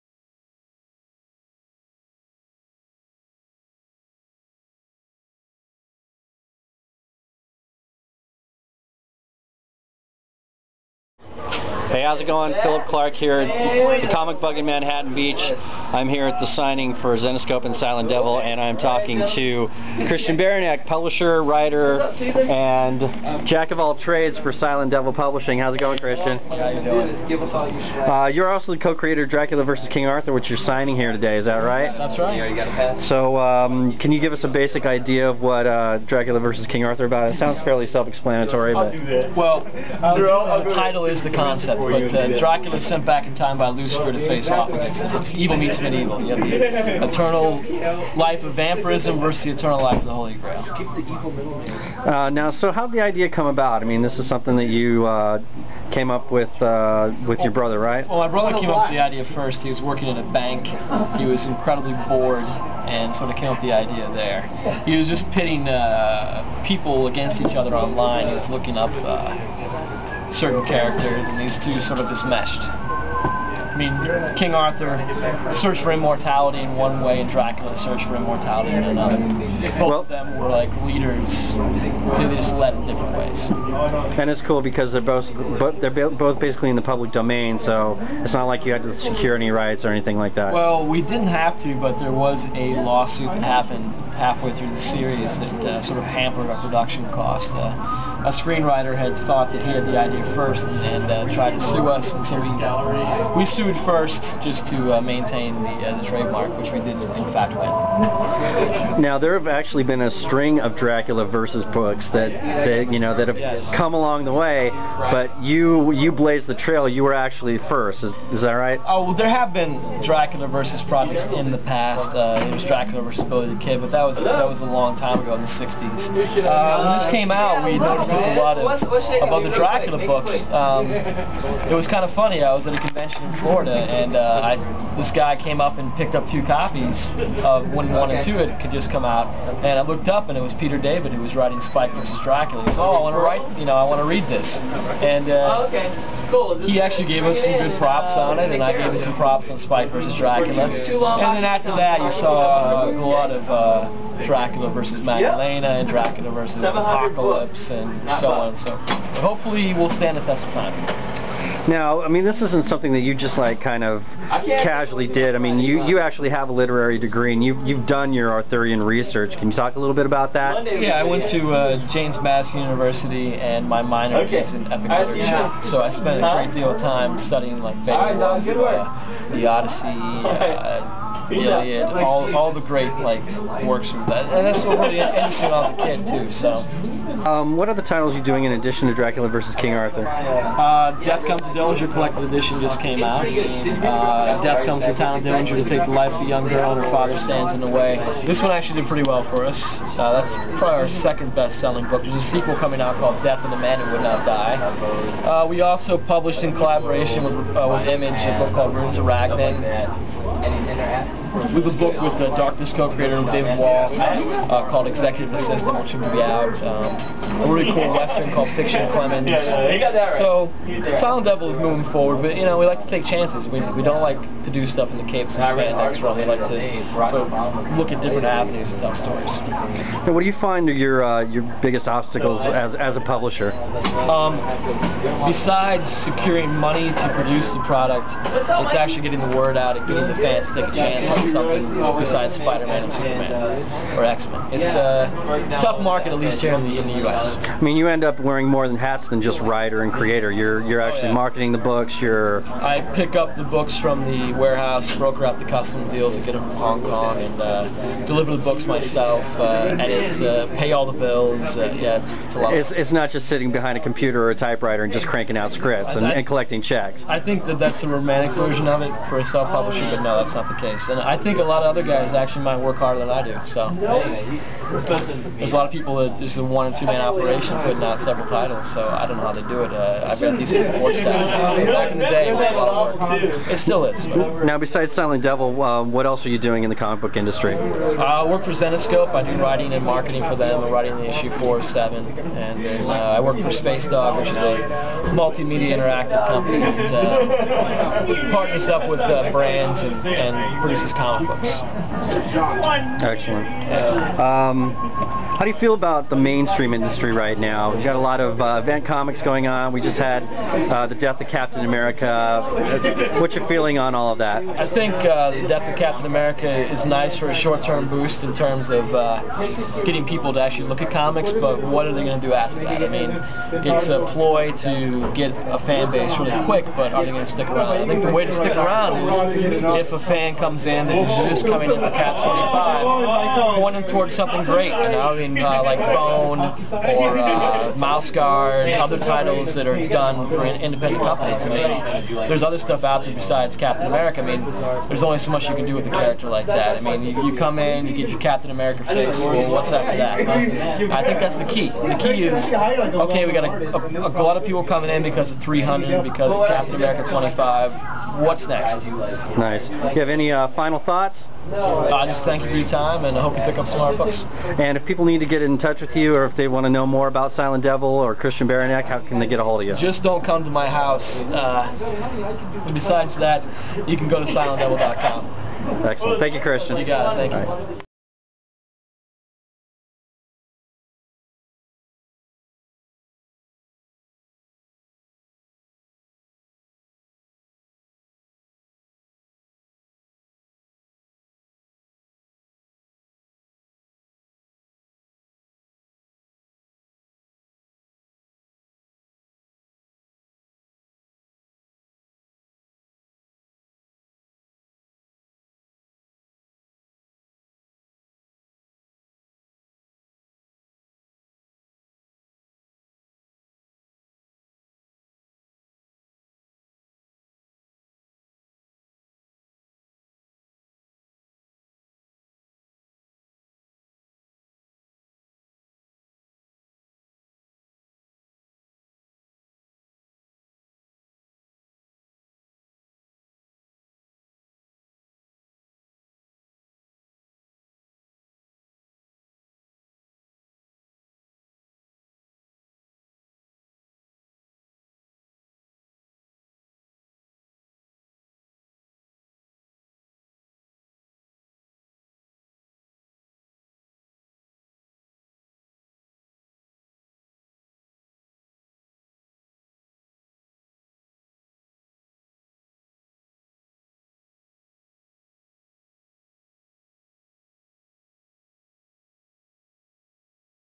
Comic Book Heroes interviews Christian Beranek
Philip Clark interviews Silent Devil publisher/writer Christian Beranek at The Comic Bug in Manhattan Beach, CA, March 2007